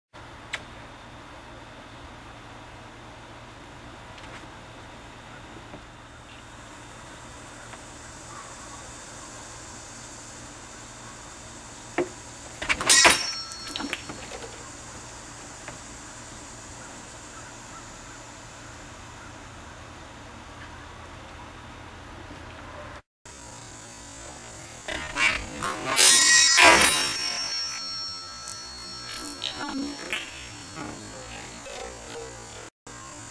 Shaolin Broadsword Opens A Bottle
Best in 720HD

The Hanwei Forge Practical Series Kung Fu Sword meets a water bottle.